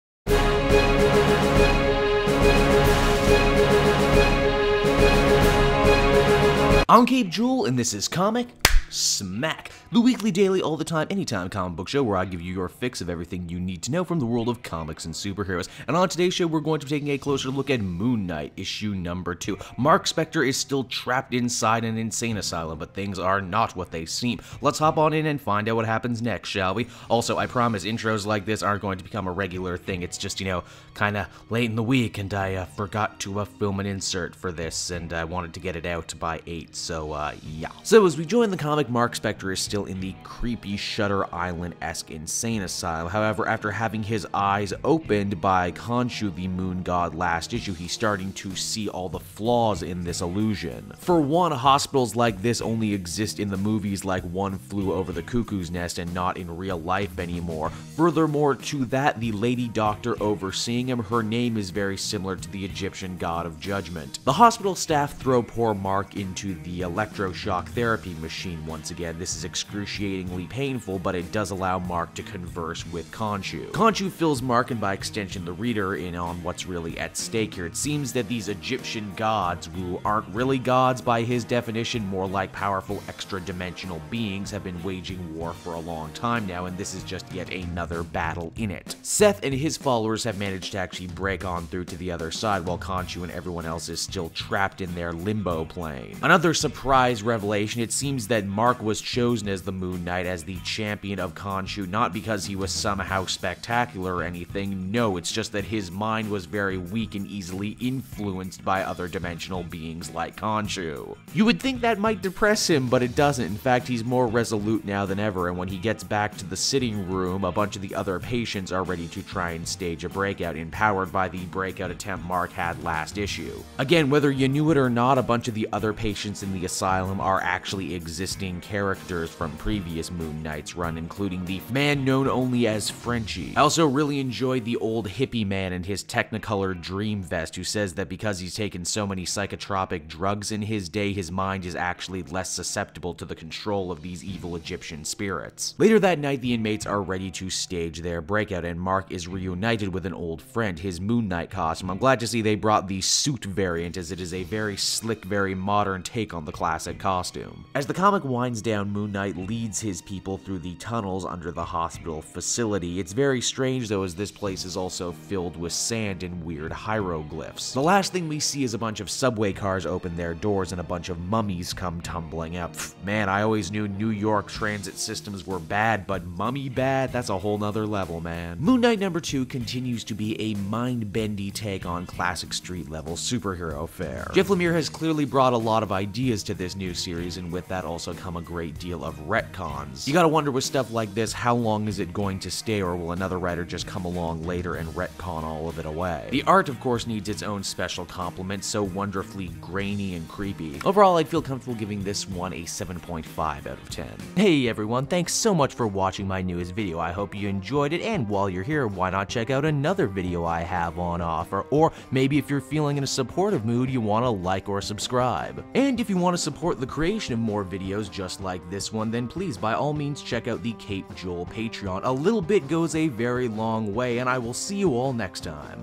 Moon Knight #2 Comic Smack
Marc Spector must escape an insane asylum if he wants to get to the truth.